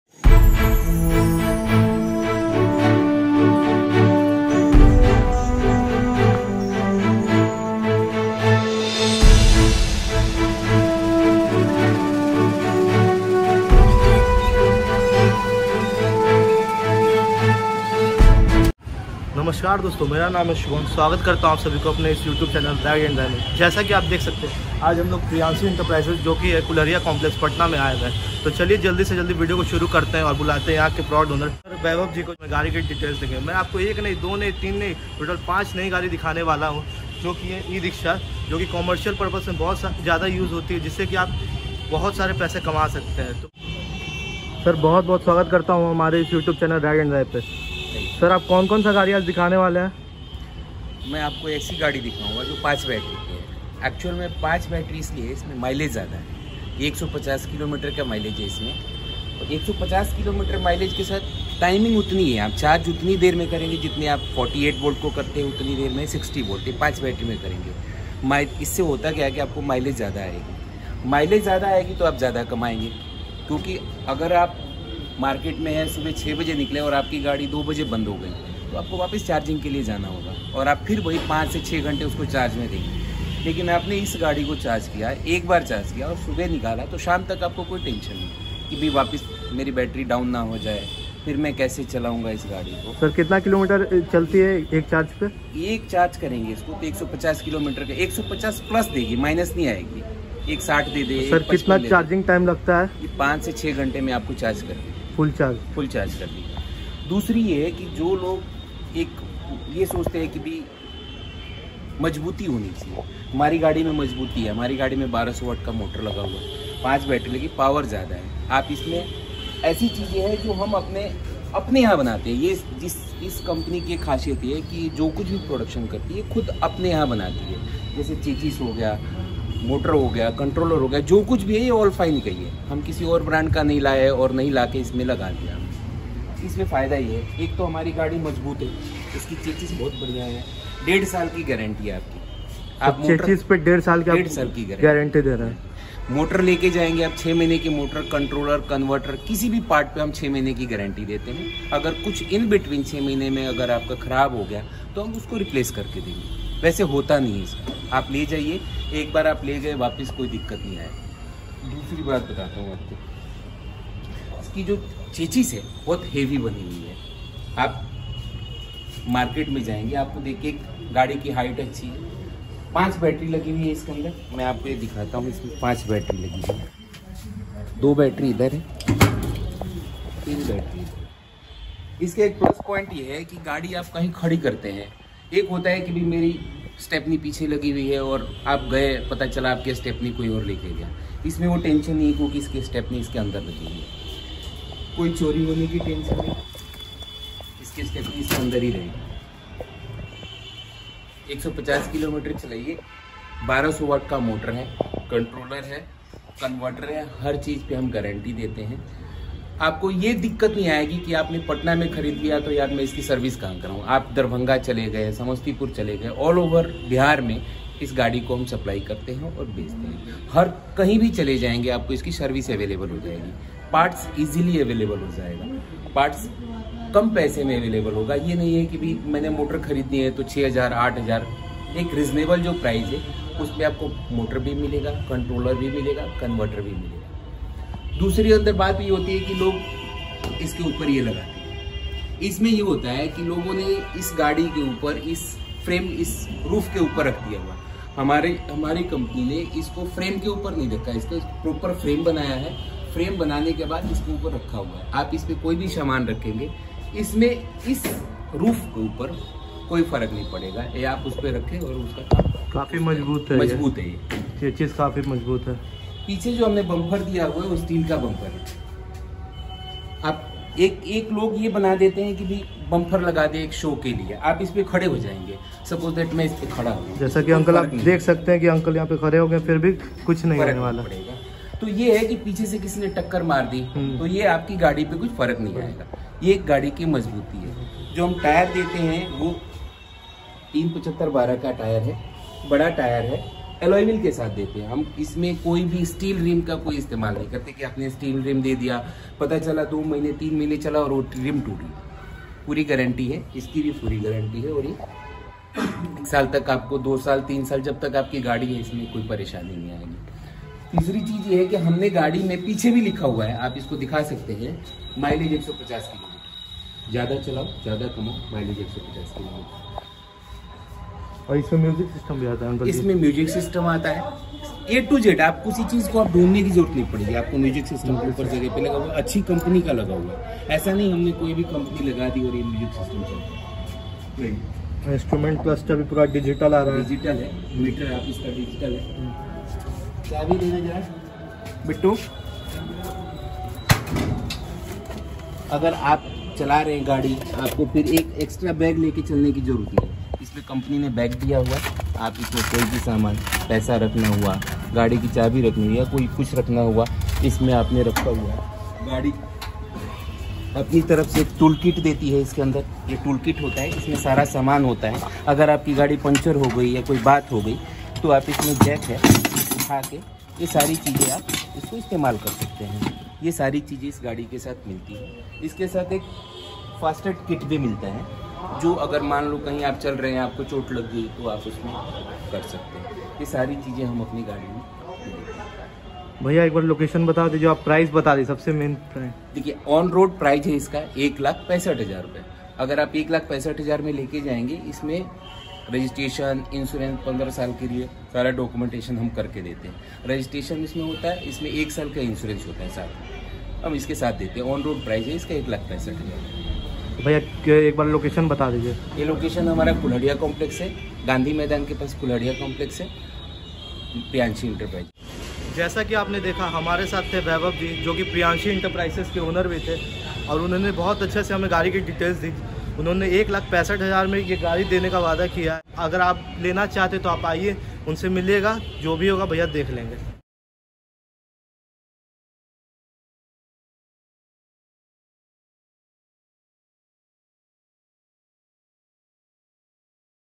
New E rickshaw shop in Patna|| एक बार Charge करे और 150km चलाये || PRIYANSHI ENTERPRISES PATNA
PRIYANSHI ENTERPRISES
Shop No.  - Kulhariya Complex Ashok Raj Path Near Gandhi Maidan Patna